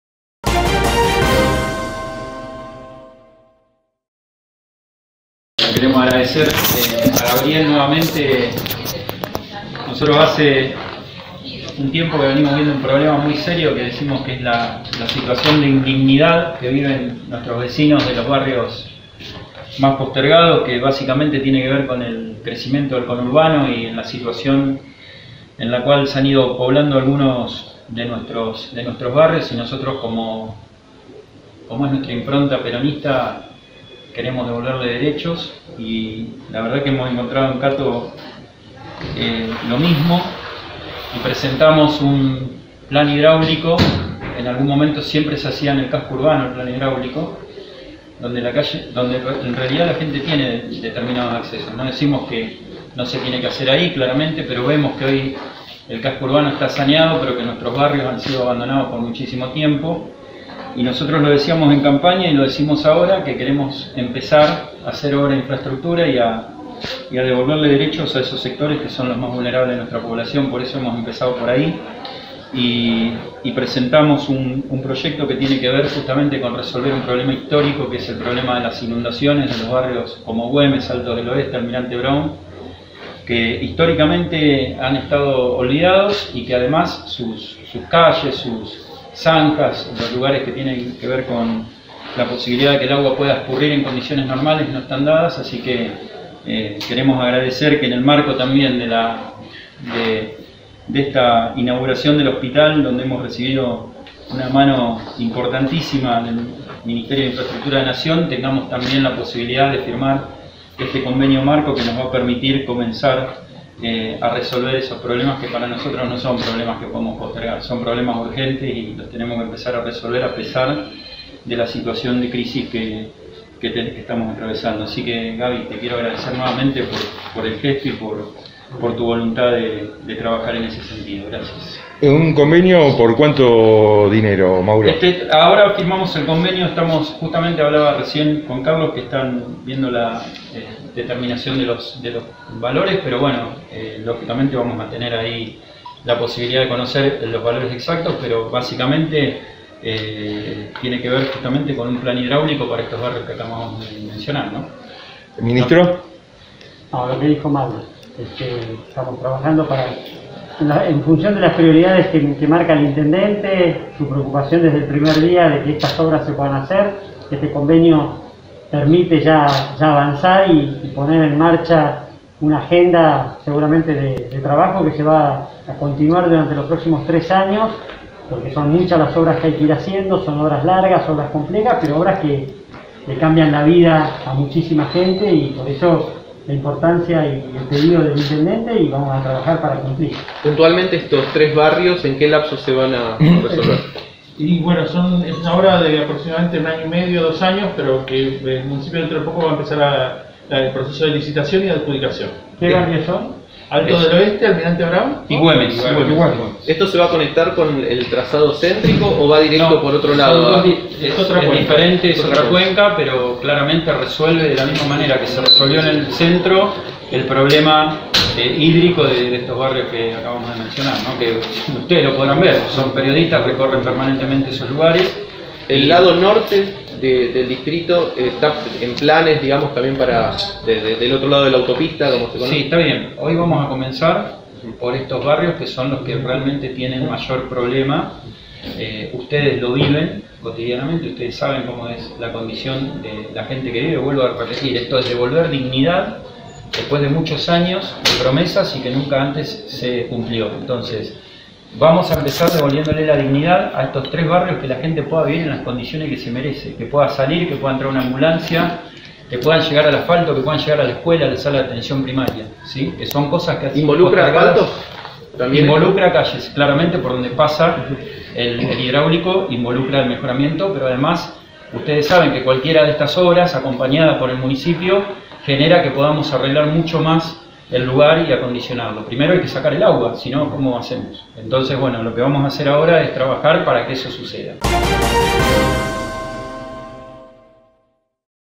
Firmaron el acuerdo para las obras de cloacas en Güemes, Altos del Oeste y Almirante Brown
Palabras del intendente municipal de General Rodríguez Mauro García y el ministro de Obras Públicas de la Nación Gabriel Katopodis